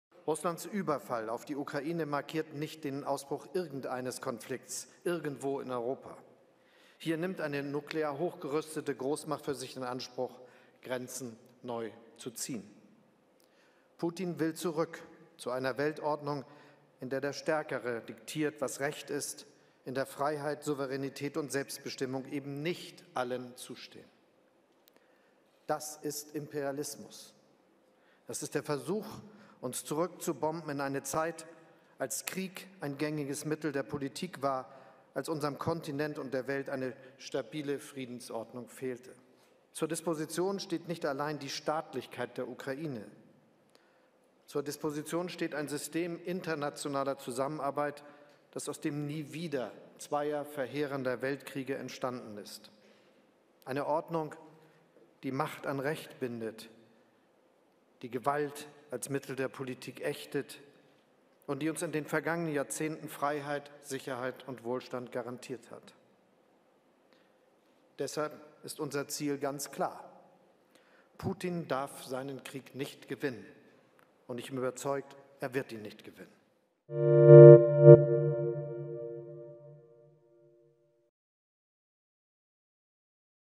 Scholz Urges Alliance Against Russia to Thwart Putin
German Chancellor Olaf Scholz on Thursday sharply condemned Russia's war in Ukraine and called for a multilateral world in which different global powers work together to overcome life-threatening challenges such as climate change, hunger and dependency on fossil energy.

Scholz, who was speaking at the World Economic Forum in Davos in Switzerland, said Russian President Vladimir Putin wants to "return to a world order in which the strongest dictate what is right."

The German chancellor described Putin’s war politics as “imperialism,” which is “trying to bomb us back to a time when war was a common means of politics, when our continent and the world lacked a stable peace order.”

Instead of using the power politics of past centuries, Scholz said it’s important all powers work together to solve threats like hunger, climate change and dependence on fossil fuels.